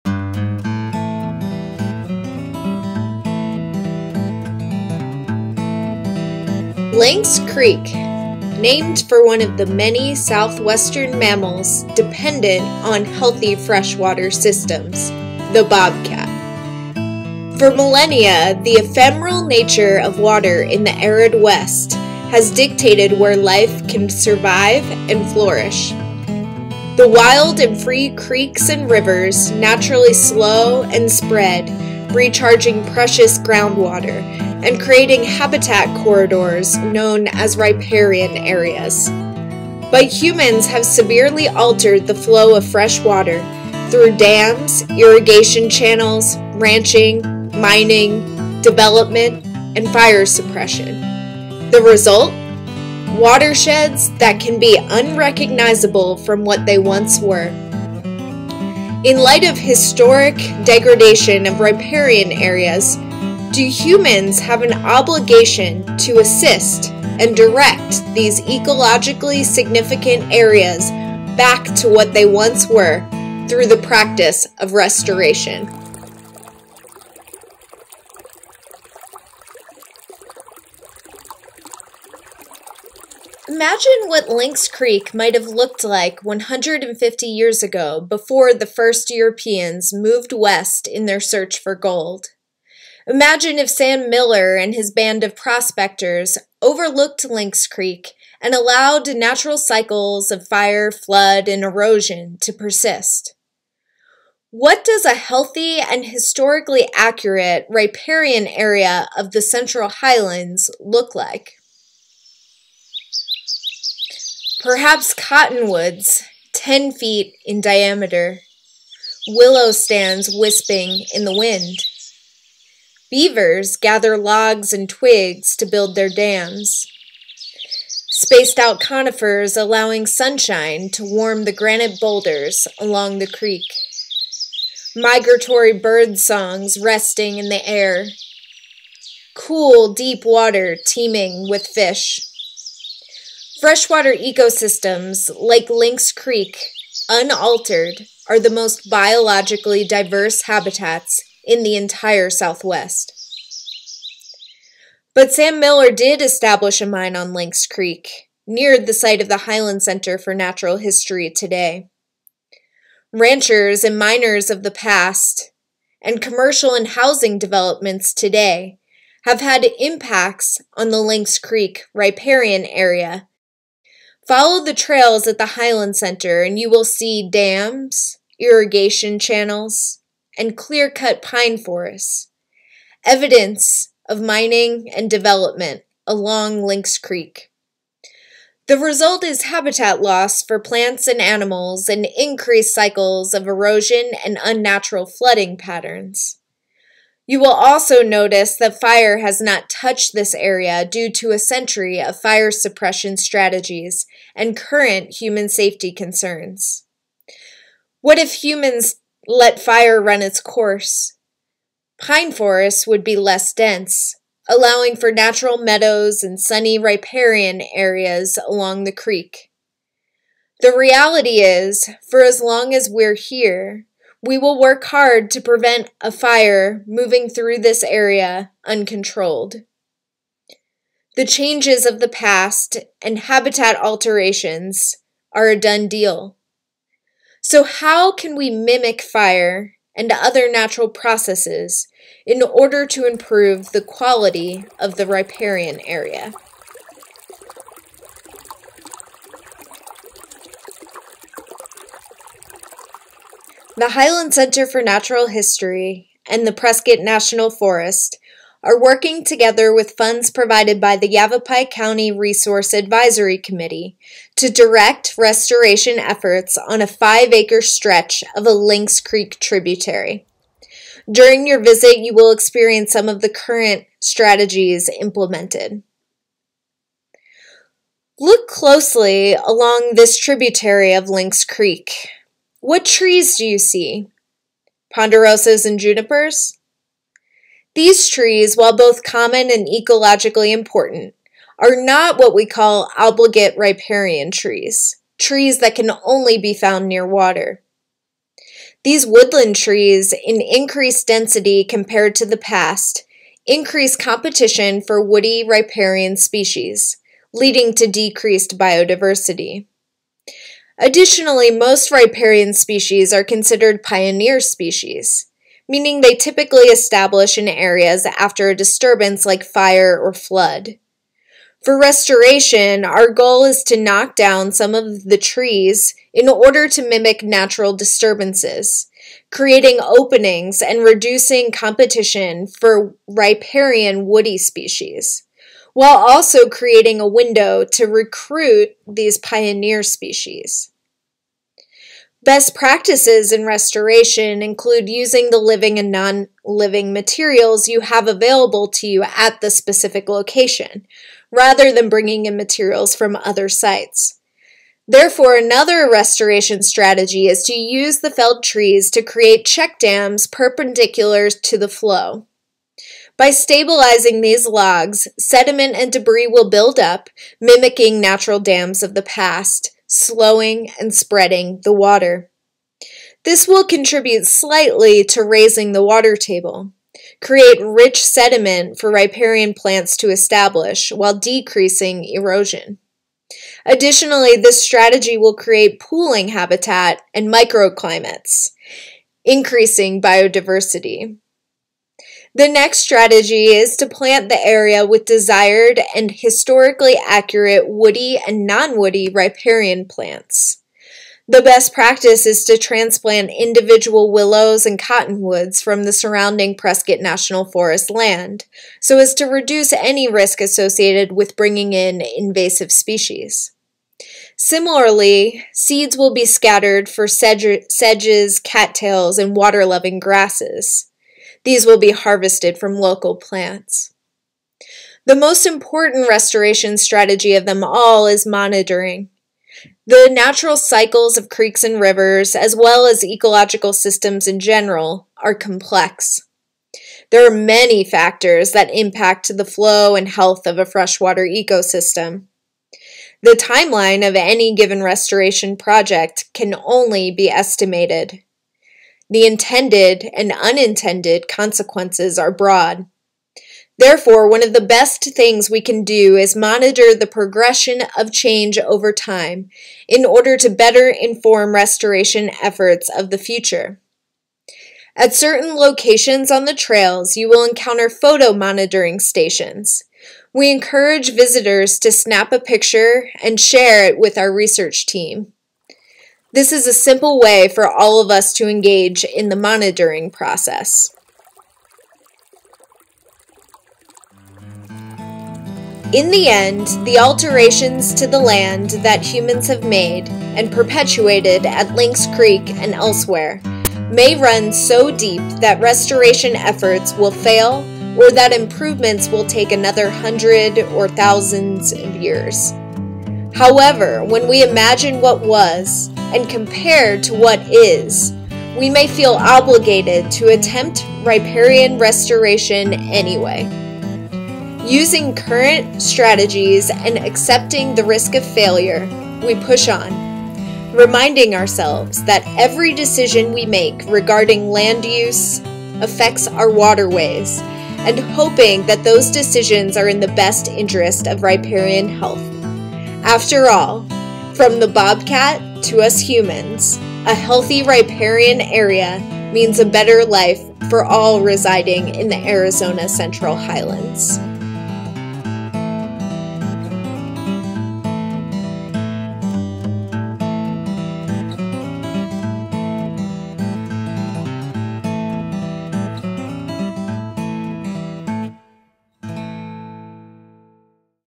Riparian Reflections: Restoration on Lynx Creek
Learn more about riparian restoration, specifically efforts made on a Lynx Creek tributary at the Highlands Center for Natural History, with funding from the Yavapai Resource Advisory Committee and in partnership with the Prescott National Forest.